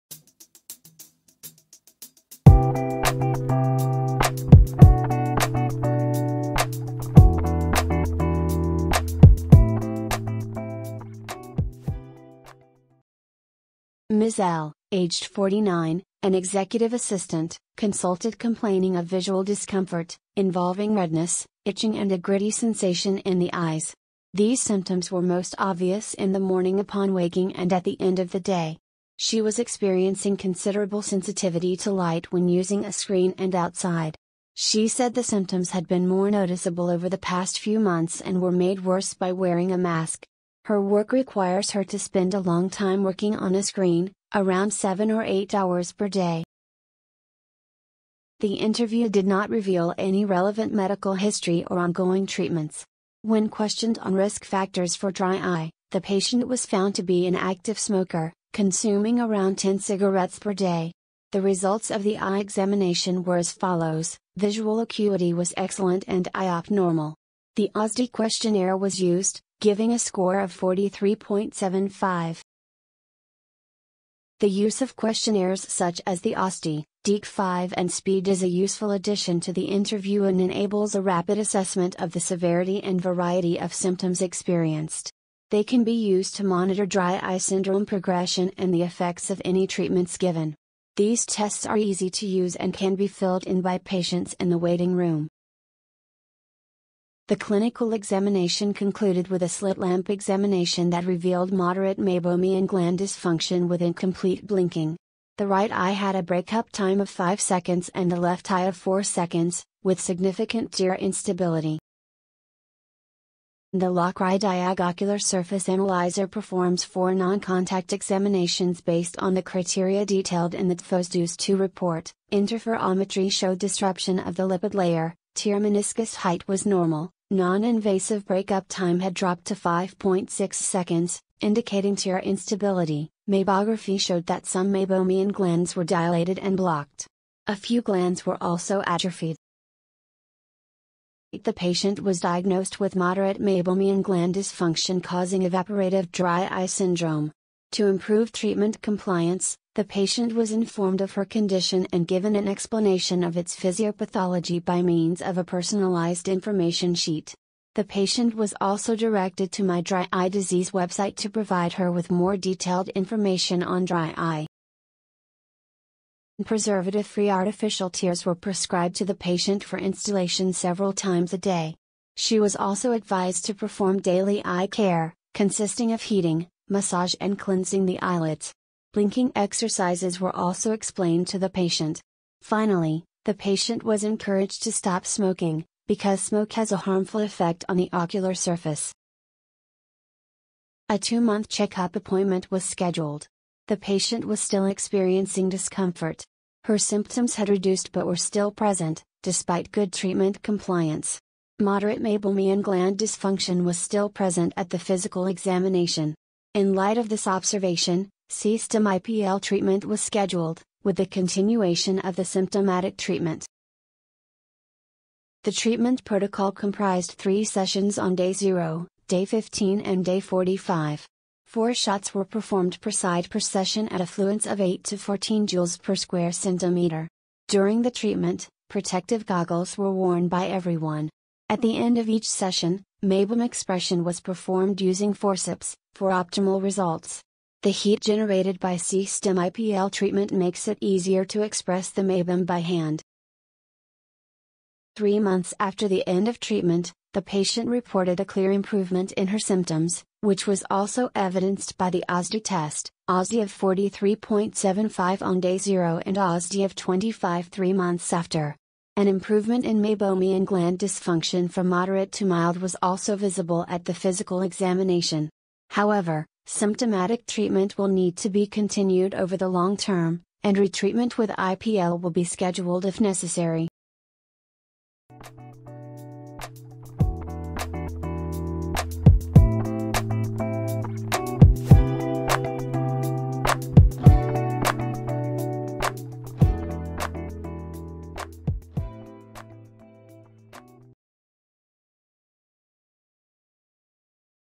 Clinical case - Moderate meibomian gland dysfunction (MGD)
Diagnosis and treatment of a female patient with evaporative Dry Eye Syndrome (DES) and moderate Meibomian Gland Dysfunction (MGD) aggravated by working at a screen and wearing a mask.
A full diagnosis was performed by means of clinical examination and LacryDiag® examinations.
C.Stim® intense pulsed light treatment was initiated in this patient. After three months, a significant reduction in symptoms was observed, with an improvement in the patient’s MGD from moderate to mild.

C.STIM® is not FDA approved.